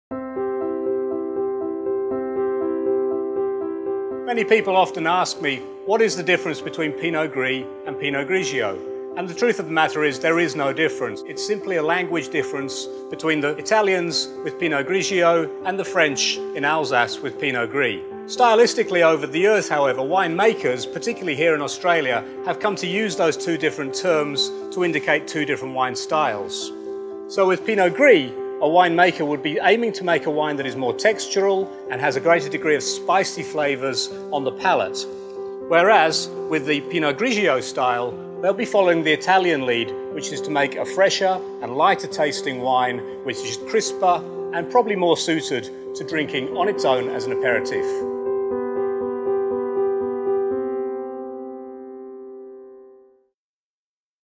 Is There a Difference Between Pinot Gris and Pinot Grigio?
Pinot Gris is intriguing. It is a white grape that makes white wine, but the variety is a mutation of the black grape, pinot noir and when ripe on the vine, the grapes are a tawny brown colour. It dates back to medieval times. It is well travelled and has gained fame in Alsace, Germany and in Italy where it is known as pinot grigio.

Watch Master of Wine Neil Hadly explain the differences between Pinot Gris and Pinot Grigio in terms of style, taste and food pairing.

To find out more about Pinot Gris and Grigio Wines, visit our wine collection at Taylors Wines.